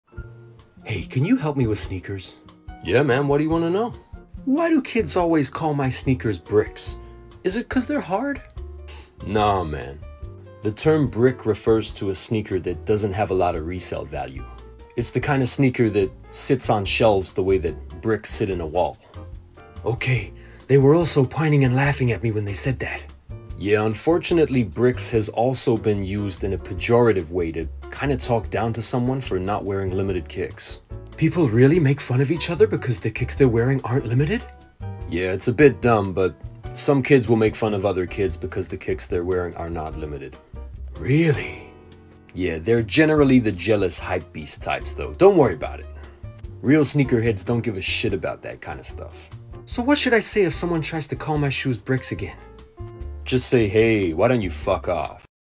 Speak sneaker head - what are bricks?
Here’s my take on the whole Kanye vs adidas beef that’s been playing out the last few months.

I’ve also included my opinion on whether I think he will go back to nike.

For my video on the Jordan 3s